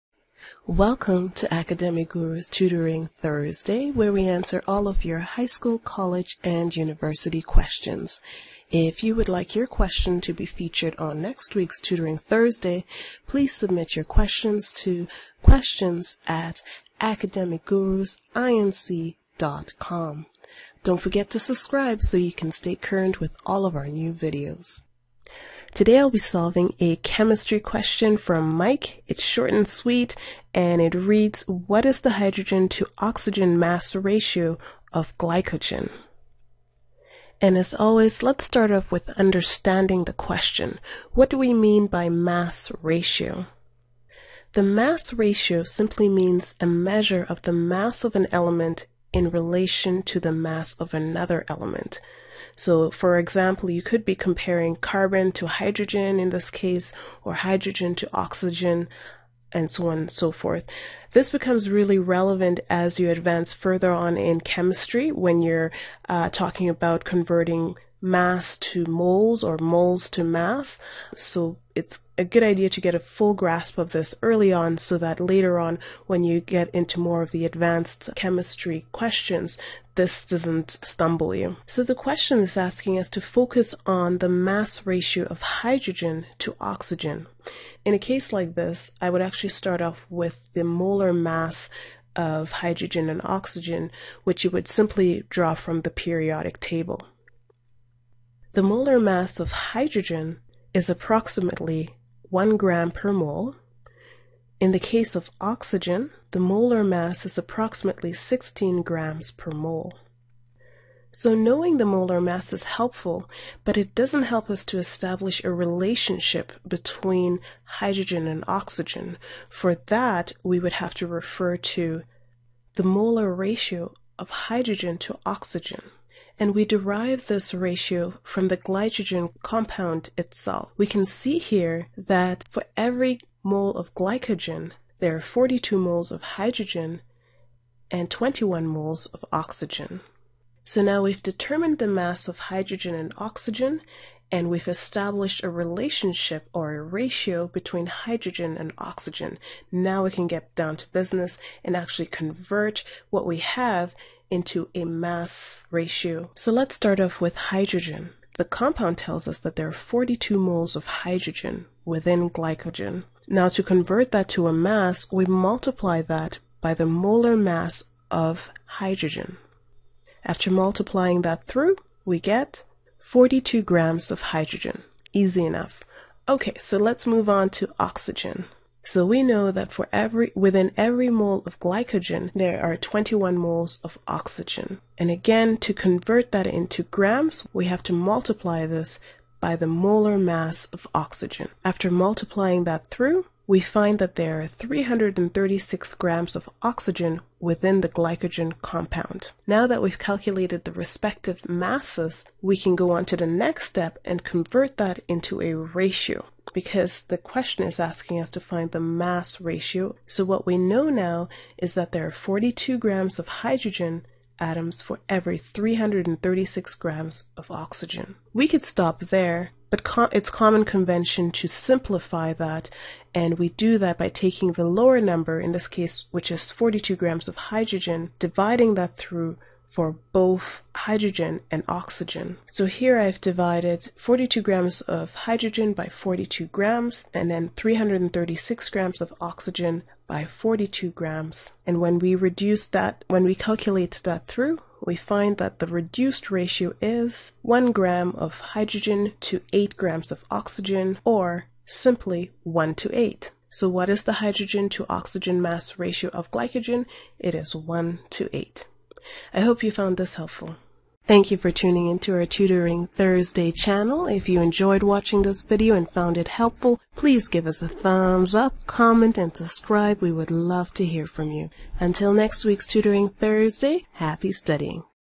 Chemistry Question: Calculating the mass ratio of a chemical compund
In this Tutoring Thursday session, we discover how to calculate the mass ratio of a chemical compound.

We demonstrate simple steps that will allow you to confidently tackle any molar-mass conversion question.